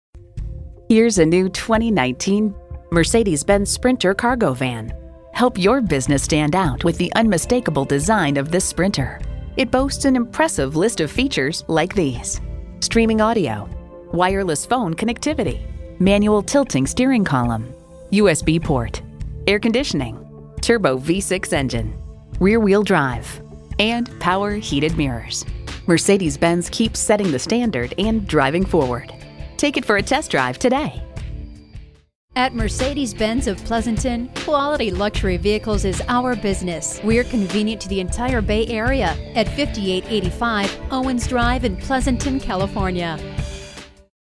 New 2019 Mercedes-Benz Sprinter Cargo Van San Francisco San Jose, CA #19-1224
Year: 2019
Make: Mercedes-Benz
Model: Sprinter Cargo Van
Engine: 6 Cyl. 3.0 L
Transmission: Automatic
Interior: Black
Stock #: 19-1224
VIN: WD3PF1CD5KP039849

Address:
5885 Owens Drive